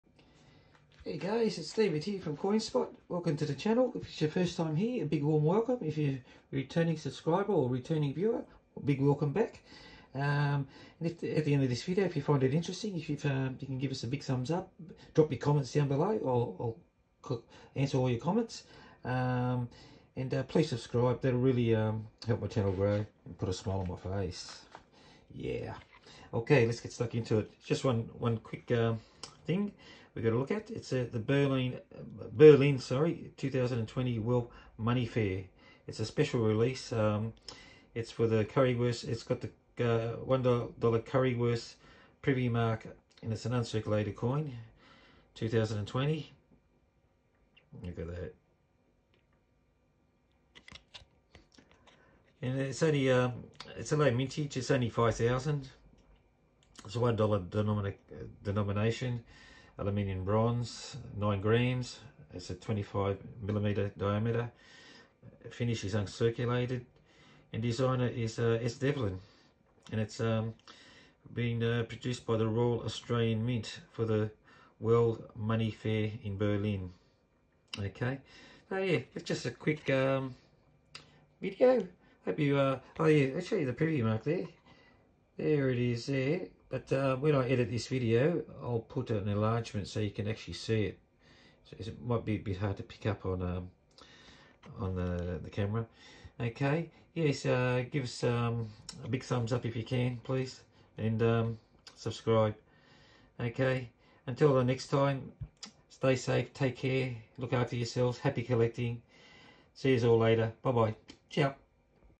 COINS|BERLIN 2020 WORLD MONEY FAIR COIN ISSUE VLOG #34
Hi Everyone
Welcome to my channel, This video is called COINS|BERLIN 2020 WORLD MONEY FAIR COIN ISSUE VLOG #34 It features $ 2020 $1 Mob of Roos coins with a special privy mark, This coin issue with the special privy mark has a mintage of 5,000.